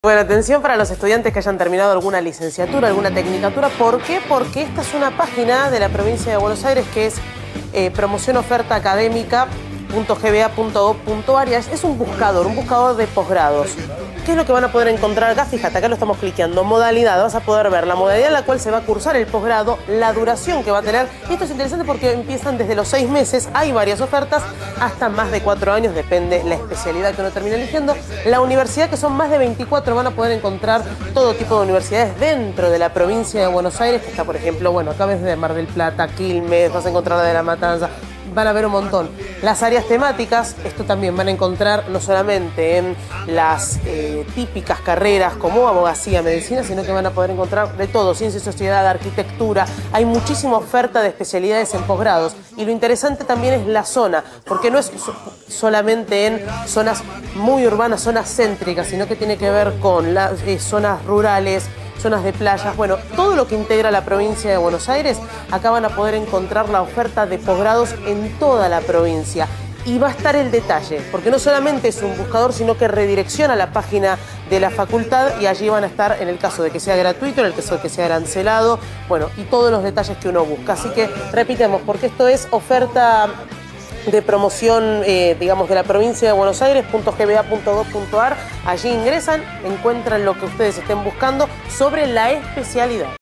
👨‍🎓Buscador de POSGRADOS y especializaciones🔍
la provincia de Buenos Aires lanza un buscador de posgrados para los estudiantes que finalizaron una carrera terciaria.